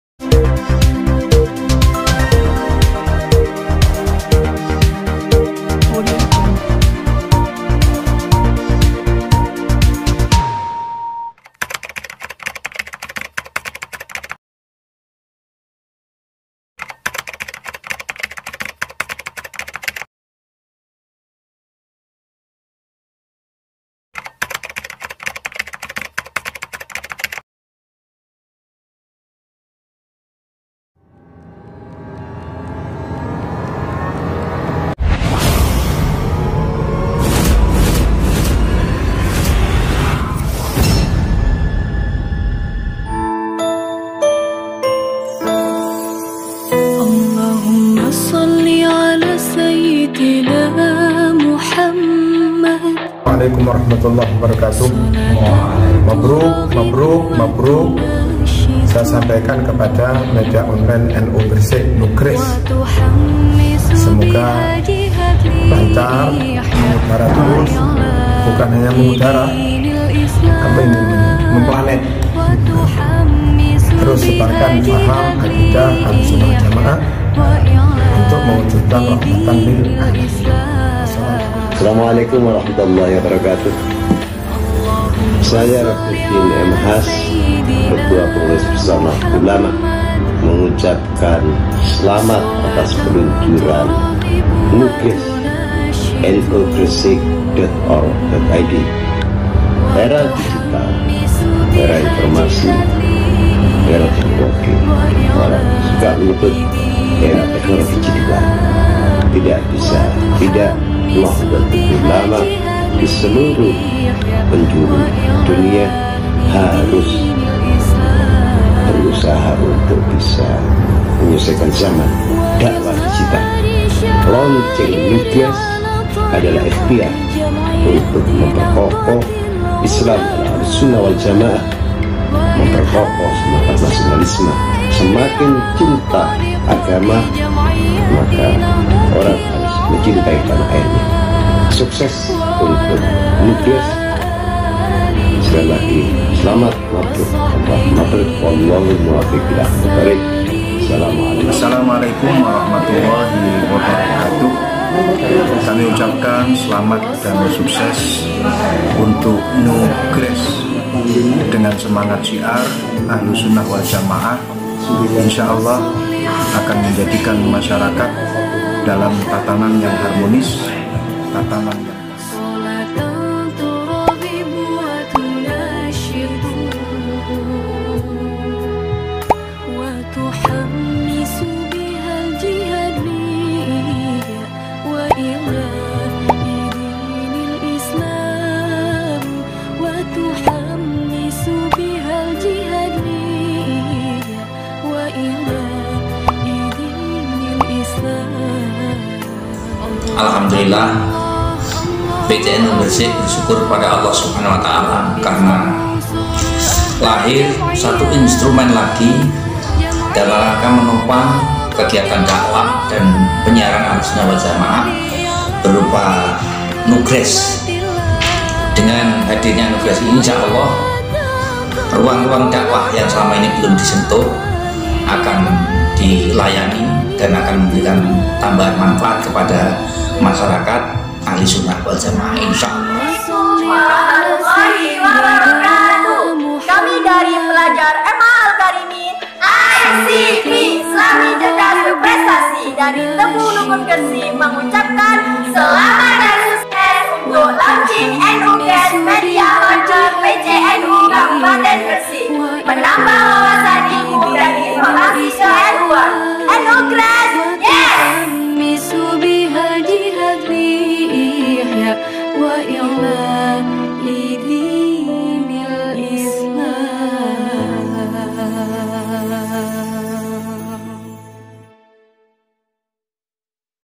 NUGRES | MEDIA CENTER PCNU GRESIK
Perkembangan zaman di era 4.0 peran teknologi digital menjadi sangat penting, begitupun di dunia dakwah terlebih bagi kaum millenial. Hal ini yang melandasi Pengurus Cabang Nahdhatul Ulama (PCNU) Gresik membuat terobosan dengan meluncurkan media center yaitu NUGres.